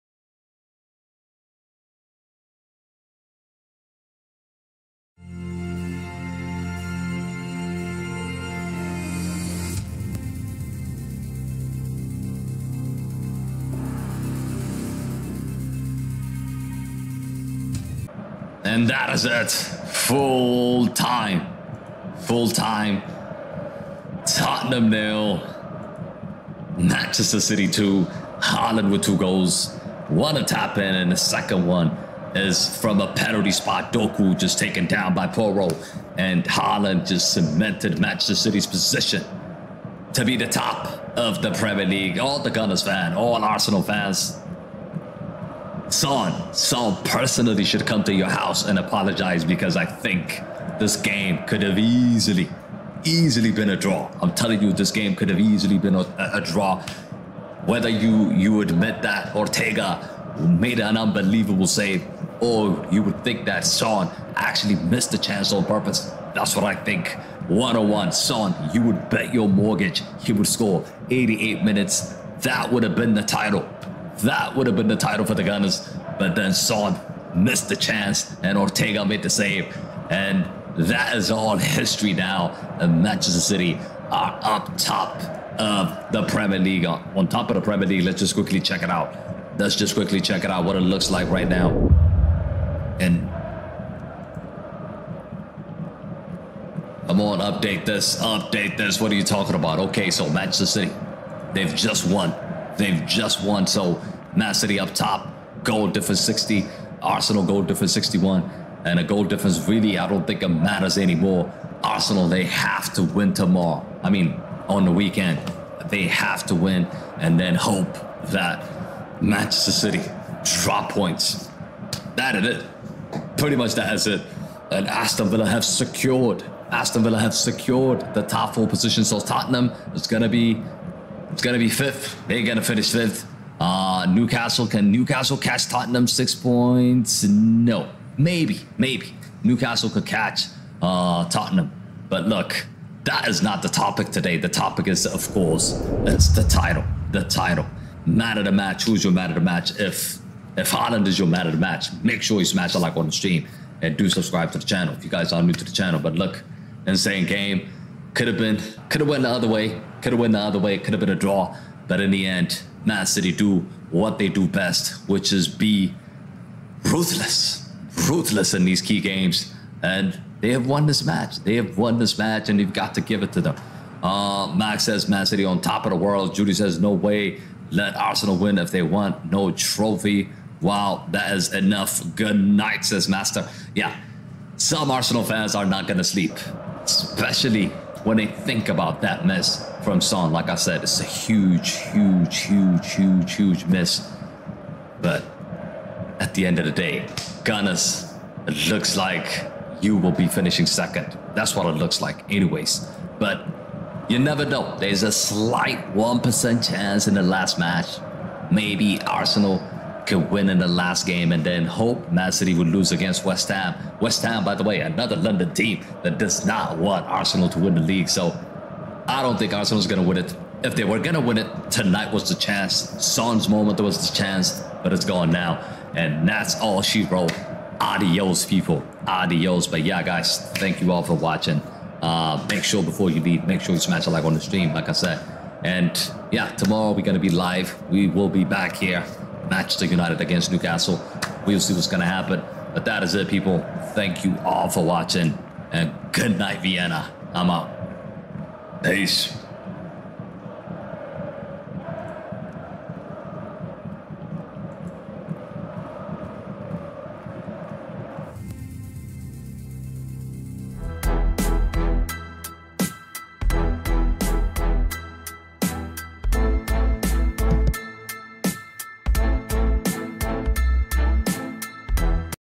Man City Go Above Arsenal! TOTTENHAM 0-2 MANCHESTER CITY
Tottenham 0-2 Man City: Erling Haaland scores twice as Pep Guardiola's side edge close to Premier League title! tottenham 0 Man City 2: Haaland double📱Download SofaScore, the best football app for stats, scores & more!